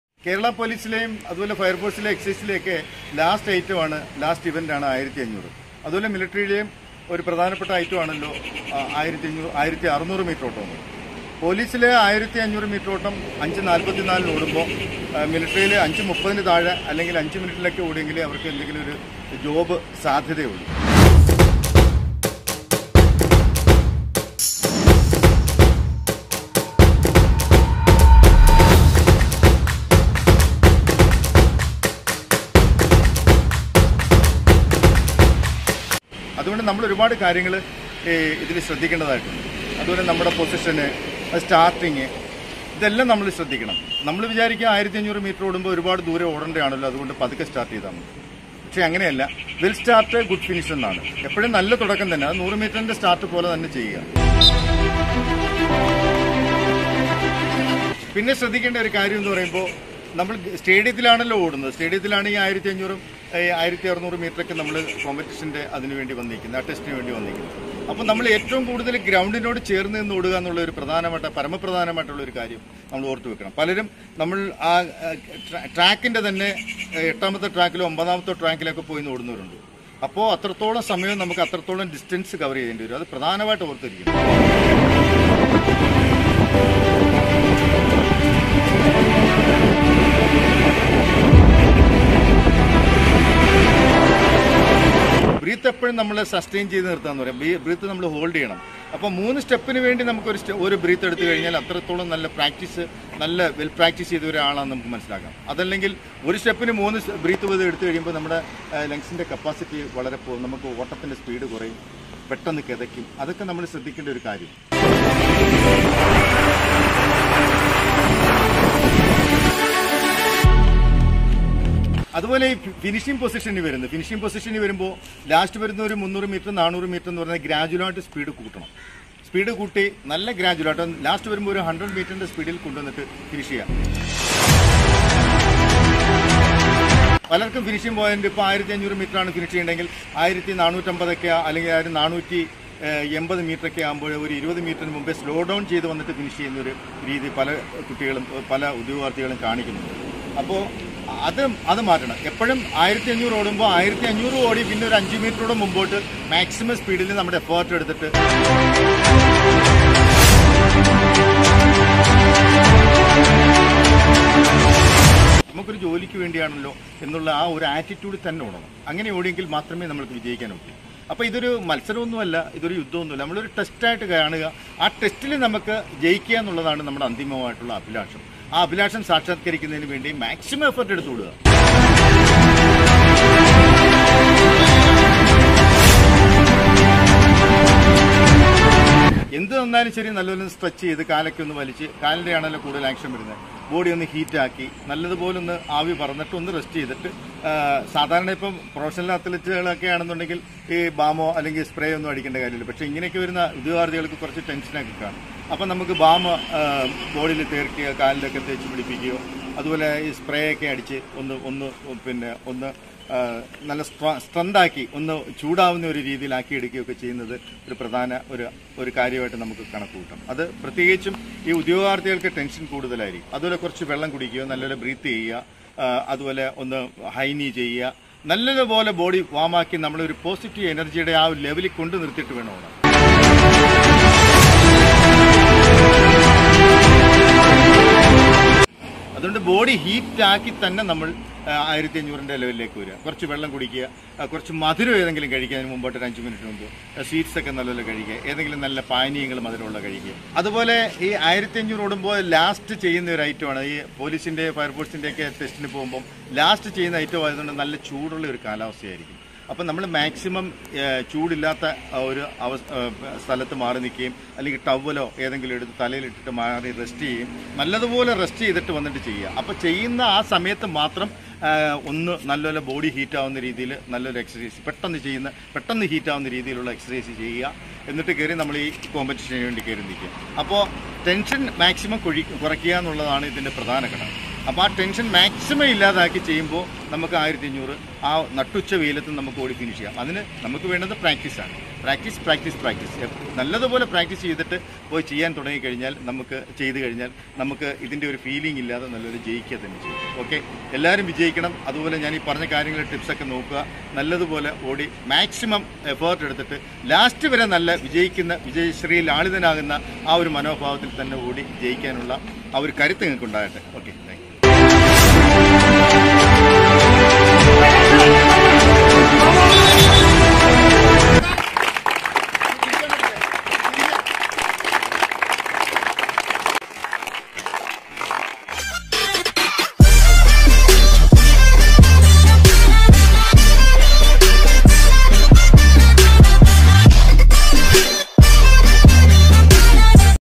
How To Run 1600/1500 Meter in 5.10 Minutes | Running Tips In Malayalam
Hai physical training academy chavara kollam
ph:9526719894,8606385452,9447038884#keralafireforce